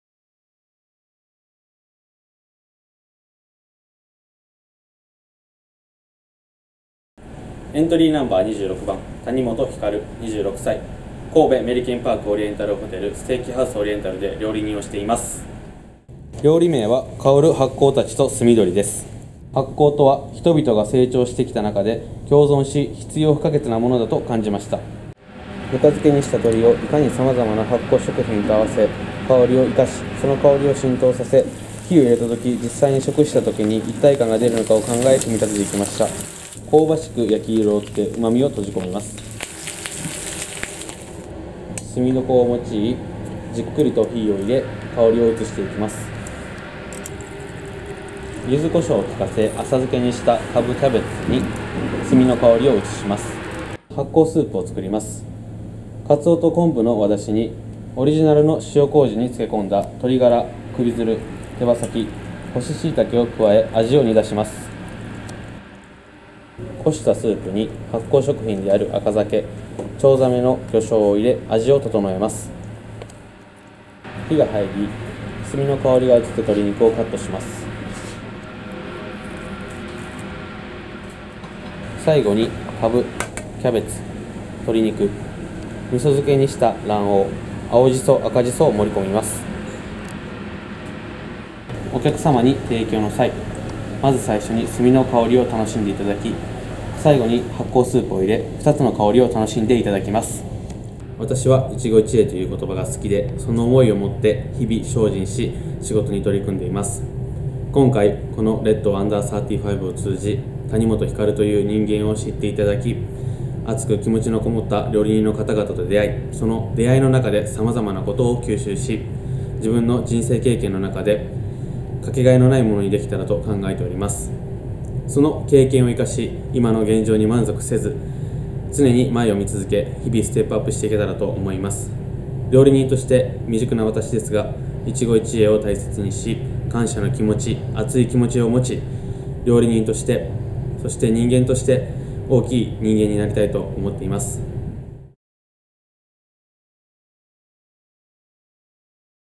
26 谷本 光（男/26）その他(鉄板焼き)「神戸メリケンパークオリエンタルホテル ステーキハウスオリエンタル」【兵庫県】
年齢 / 26
出身地 / 兵庫県
調理経験年数 / 6年
専門ジャンル / 鉄板焼き
店舗名 / 神戸メリケンパークオリエンタルホテル ステーキハウスオリエンタル
店舗所在地 / 兵庫県
店舗業態 / ホテル・旅館
料理スタイル / 旬を捉え一体感のある料理